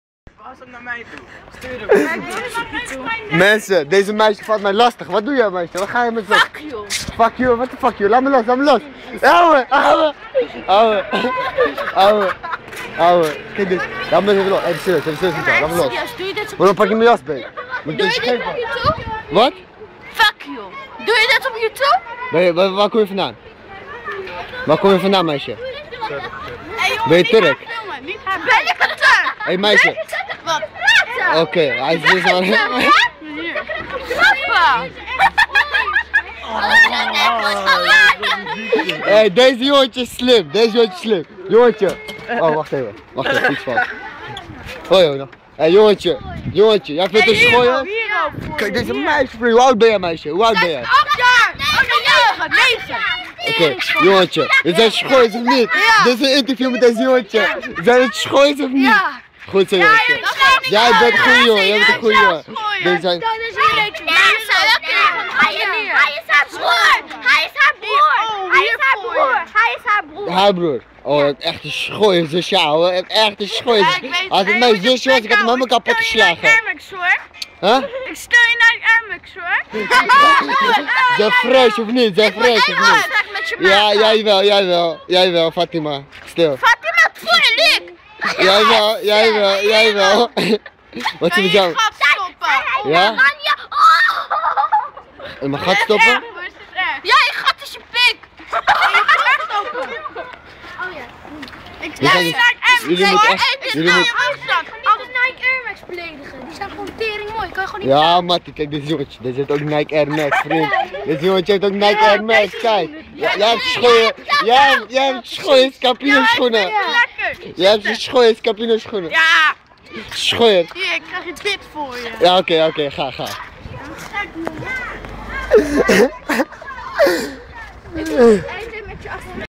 gekkekinderen2
deel 2 je weet zelf ze vechten om voetbal kaartjes